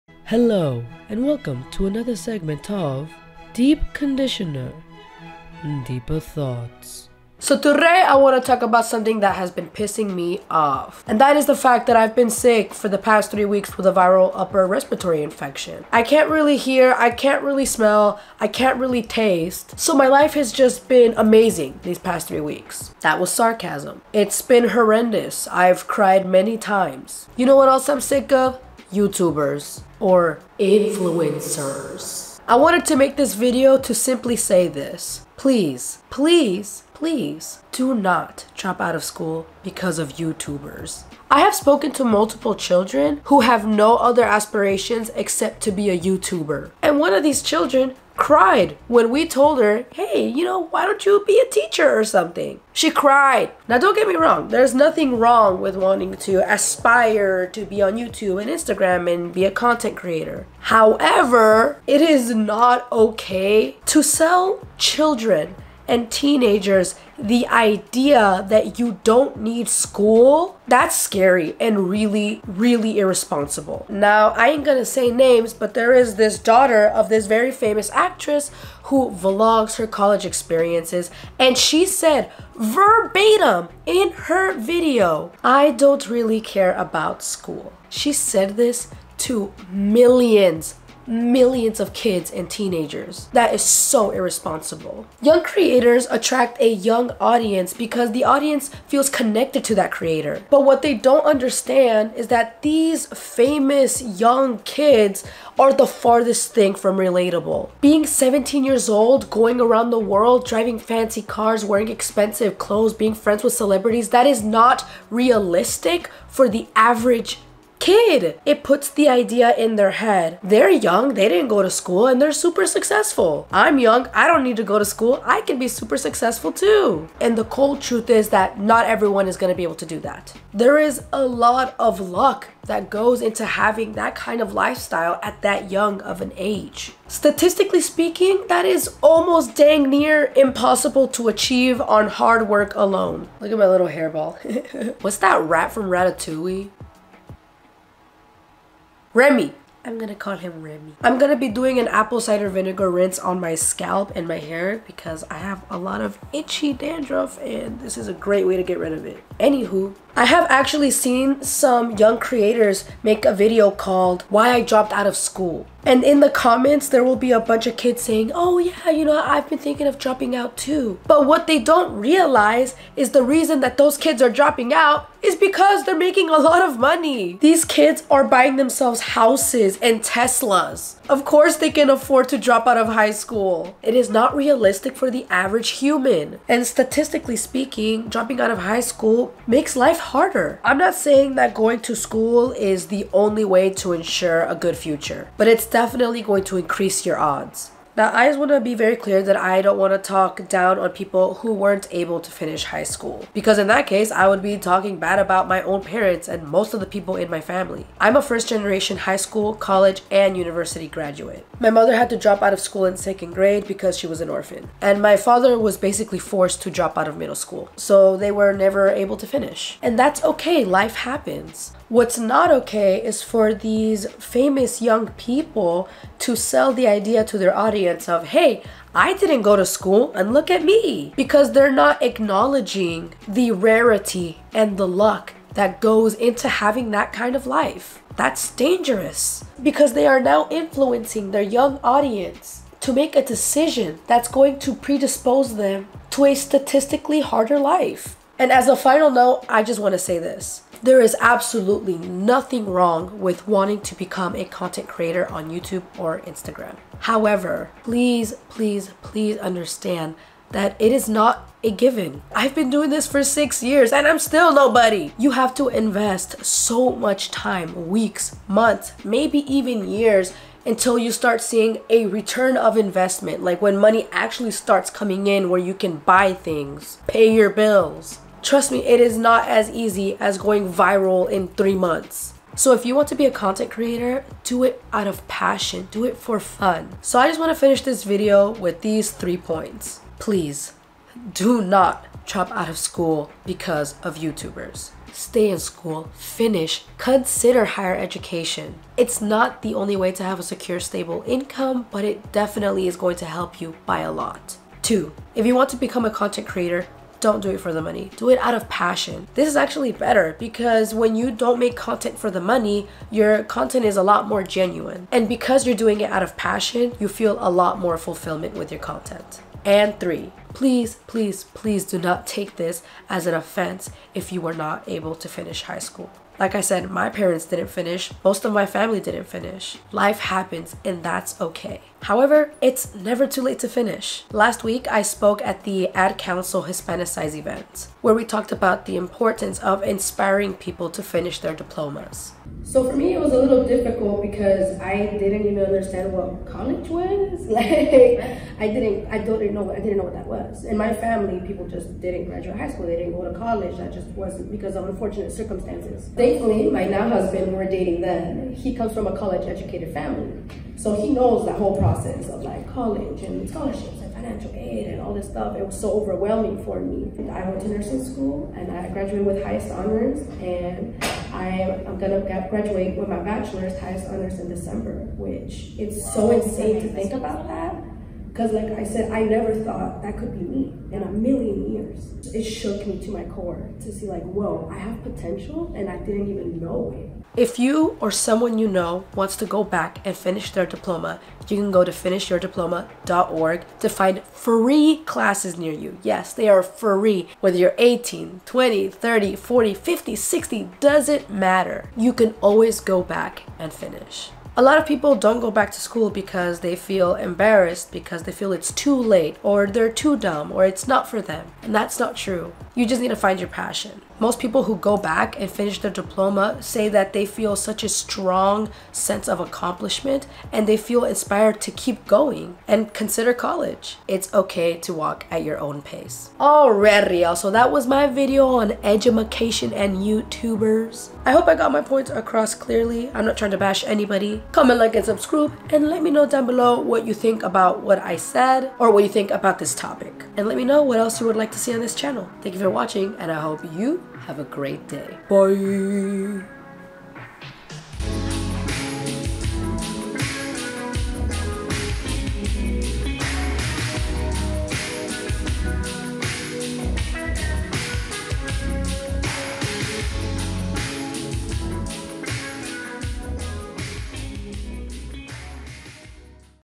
Dear YouTubers, stop making kids drop out of school
Today's DEEP I talk about how youtubers, influencers, and social media encourage kids and teenagers to drop out of school and not pursue higher education.

Roughly 34 million adults in America don't have their high school diplomas. To learn more about tests and free classes in your area, visit FinishYourDiploma.org :)